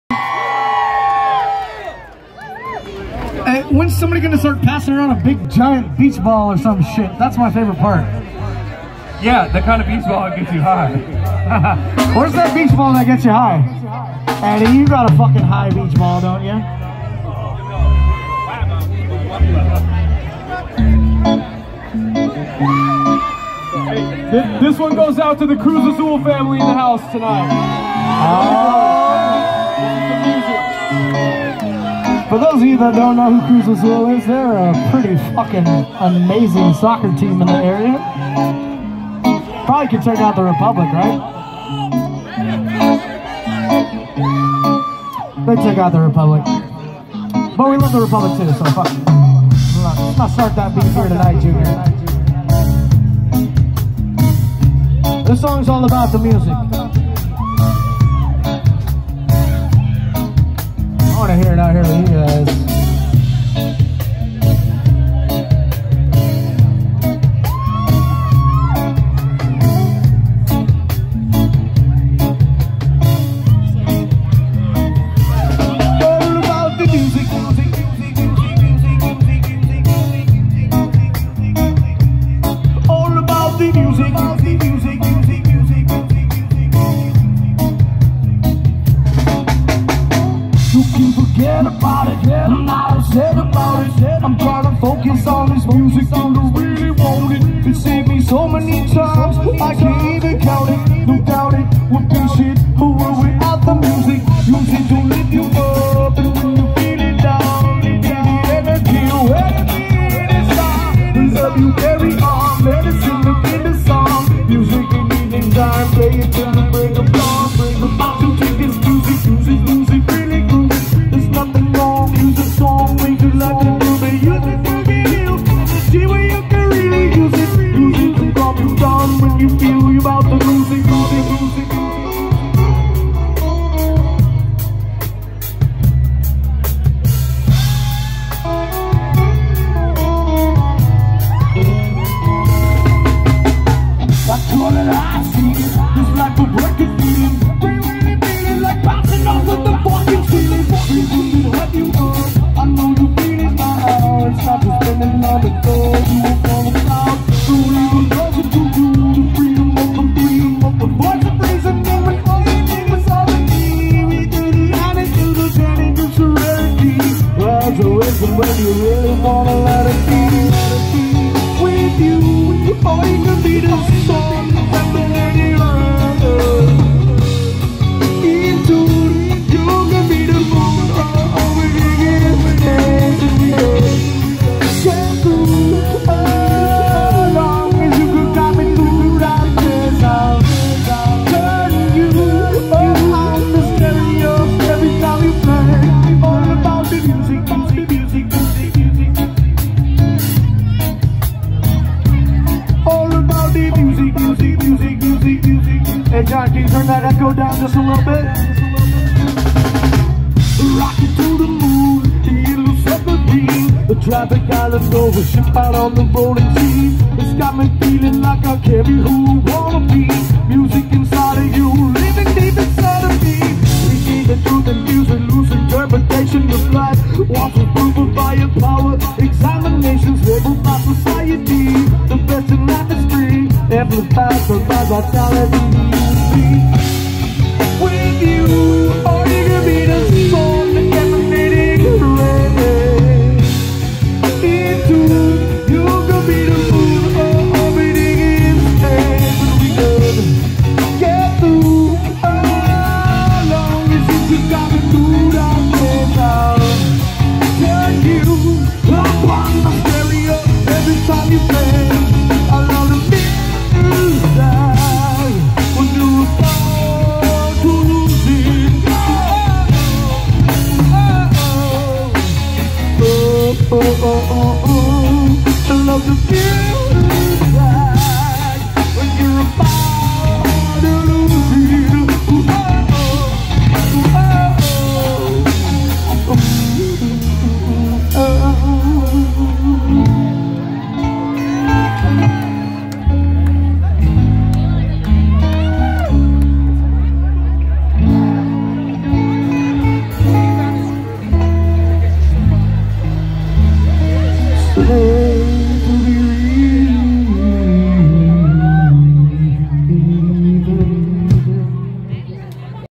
Arden Park Roots K Street 7/23/2021
Arden Park Roots performing The Music on K Street Sacramento.